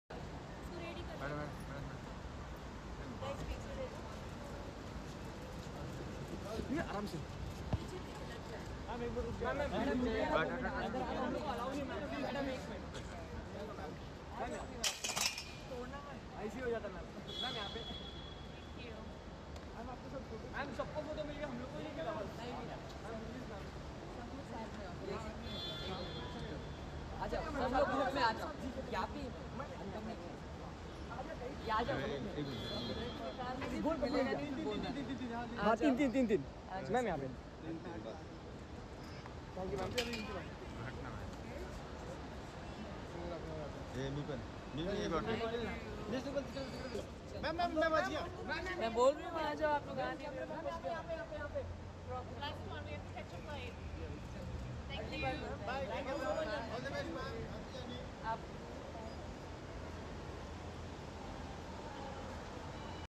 Priyanka Chopra Aces Airport Look In Comfy Co-Ord And Oversized Jacket!
Global icon Priyanka Chopra Jonas was spotted at Mumbai airport on Saturday. She looked chic in a comfy co-ord set, topped off with an oversized jacket, a cap, white shoes, and stylish shades. She completed her look with a trendy tote bag. Watch the video to get a glimpse of her look!